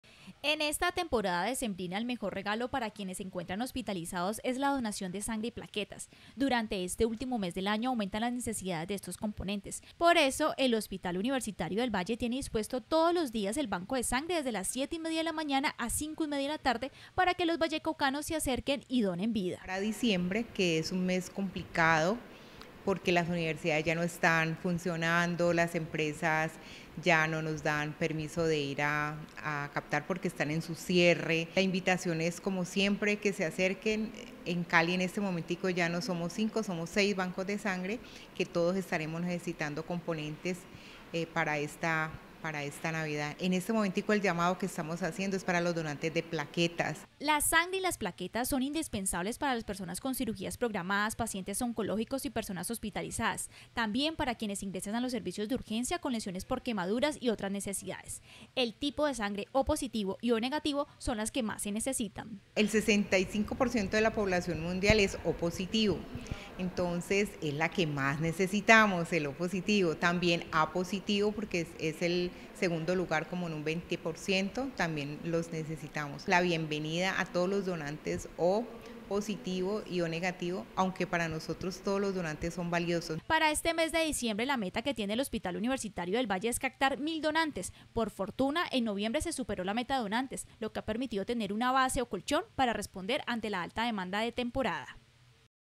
Vallecaucanos, invitados a donar sangre y plaquetas en el HUV
Emitido el  16 de  diciembre 2023 dia  por el Canal regional Telepacífico.

En temporada decembrina el mejor regalo para quienes se encuentran en hospitales y clínicas puede ser la donación de sangre y plaquetas. El Hospital Universitario del Valle tiene dispuesto todos los días el Banco de Sangre de 7:30 a.m. a 5:30 p.m. para recibir la donación de estos componentes que se requieren permanentemente, pero en especial en este último mes del año cuando en medio de las celebraciones aumentan las necesidades.